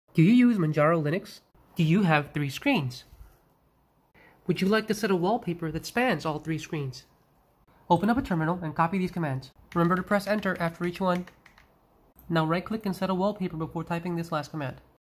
Triple screen wallpaper on Arch Linux Manjaro! Superpaper!
The shortest explanation possible on how to setup superpaper on Arch Linux in this case Manjaro Linux latest version 2022.